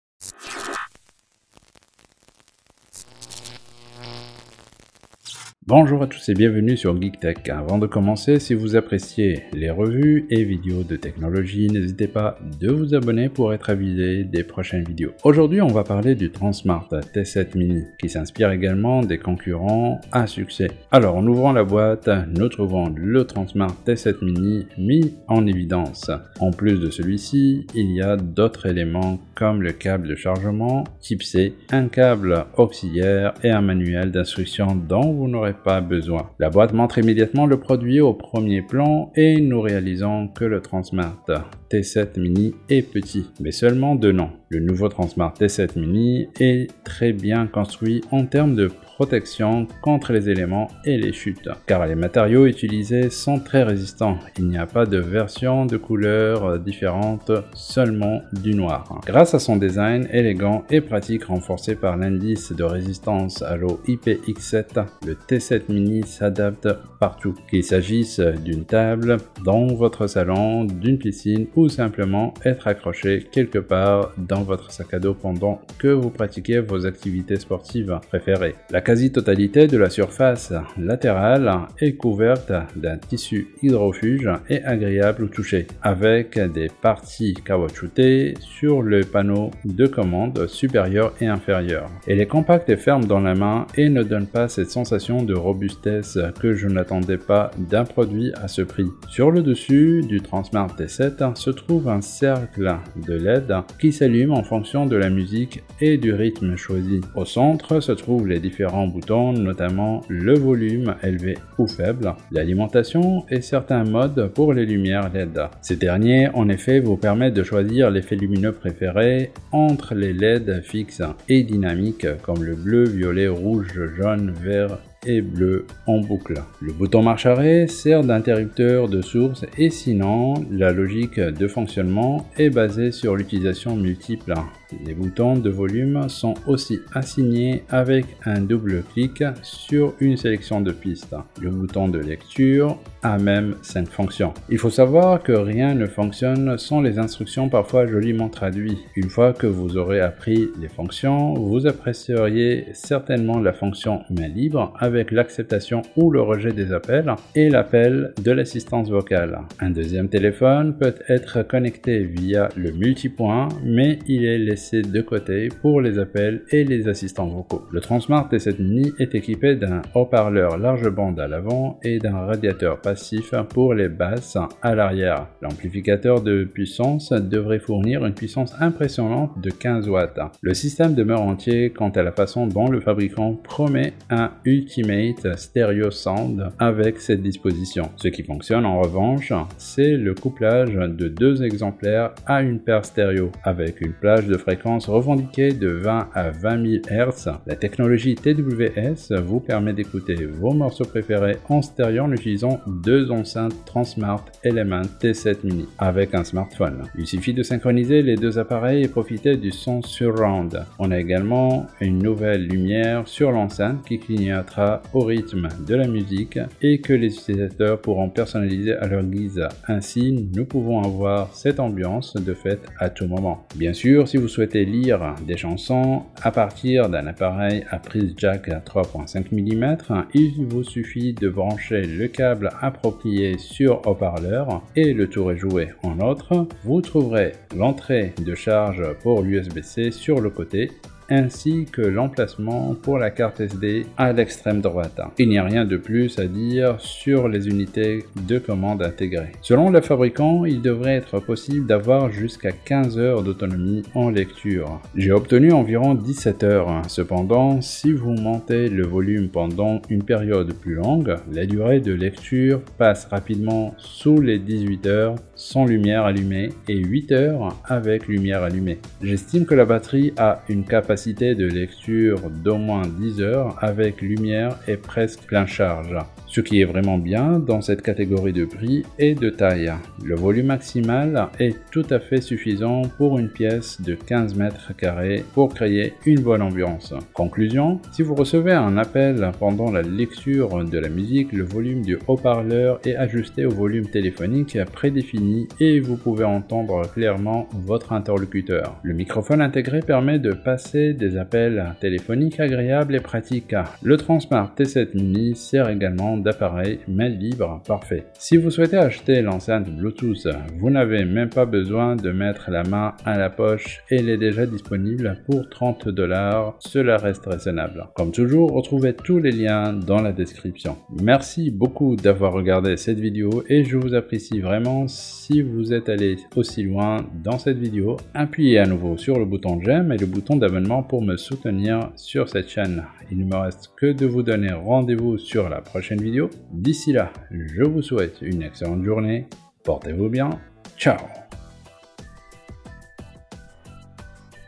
🔥 Haut parleur Tronsmart T7 Mini UNBOXING & Ses fonctions
🔥Nouveau mini haut-parleur Tronsmart T7 mini 😮👌Je vous montre toutes ses fonctions détaillées avant de l'acheter 😃Batterie de 18 heures.
Ce nouveau haut-parleur est très agréable et robuste, qui se sent très à l'aise dans la main, le son est très équilibré et de grande qualité uniquement dans certaines chansons à plein volume avec beaucoup de basses légèrement déformées, mais pour son prix ça C'est environ 30 dollars, est ça vaut le coup, mini haut-parleur Bluetooth portable Tronsmart T7 mini

Lien @TronSmartYTB Mini